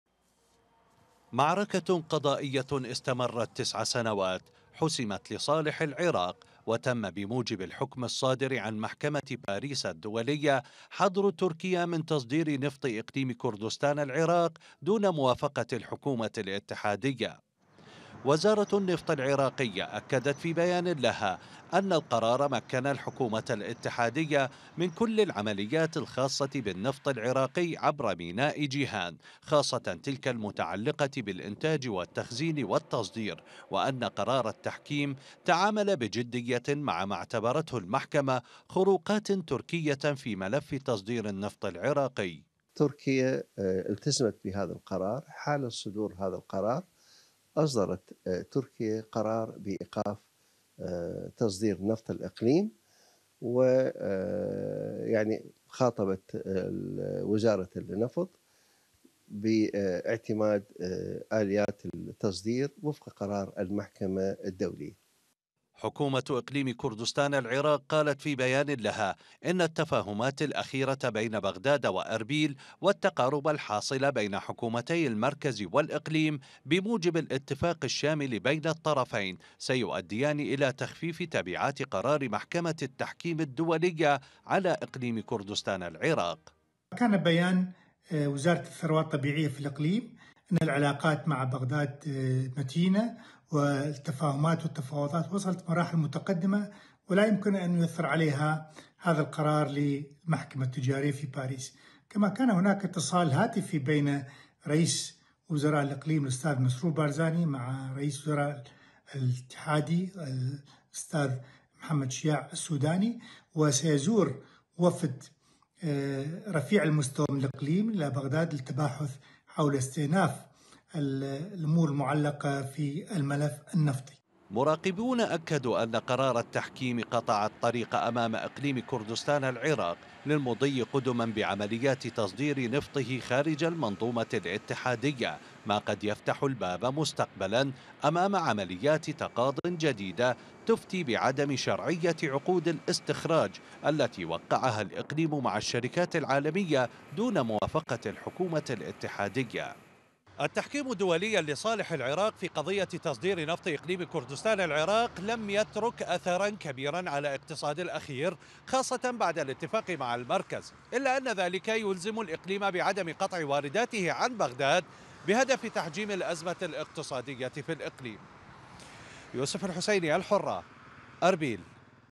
وفد من إقليم كردستان في بغداد بعد قرار محكمة باريس الدولية المتعلق بقضية تصدير النفط عبر تركيا
ترأس وزير النفط حيان عبد الغني، اجتماعا مع وفد إقليم كردستان بعد قرار محكمة باريس الدولية، ووفقاً لبيان وزارة النفط، فقد بحث الجانبان الآليات والمعطيات الجديدة لصادرات الإقليم بعد القرار.

حمل تطبيق "الحرة كونيكت" للتلفزيونات والهواتف الذكية:

أمازون فاير